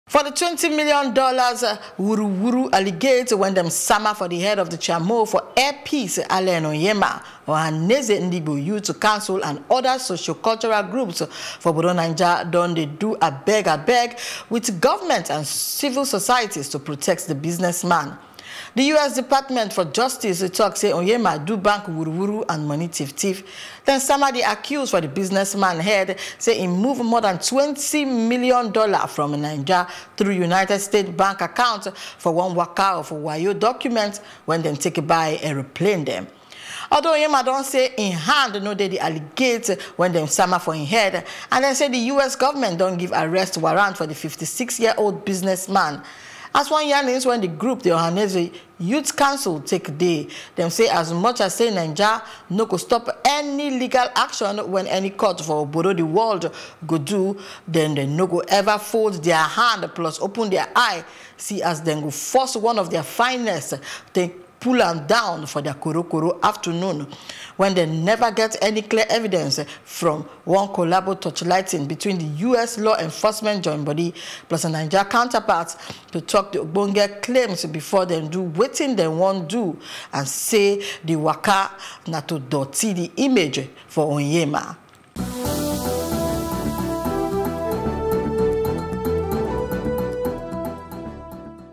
$20M Wuruwuru: Ohaneze, Socio-Cultural Join Body Tell FG To Protect Onyema
For the $20m wuruwuru allegate wen dem sama for de head of the Chairmo of Air Peace, Allen Onyema, Ohanaeze Ndigbo Youth Council and other socio-cultural groups for Naija don dey doo abeg abeg with government and civil societies to protect the businessman.

The US Department of Justice tok say Onyema do bank wuruwuru and money tif tif.

Dem sama de acuse for the businessman head say he move more than $20 million from Naija through United States bank accounts for wan waka of wayo documents wen dem take buy airoplan dem.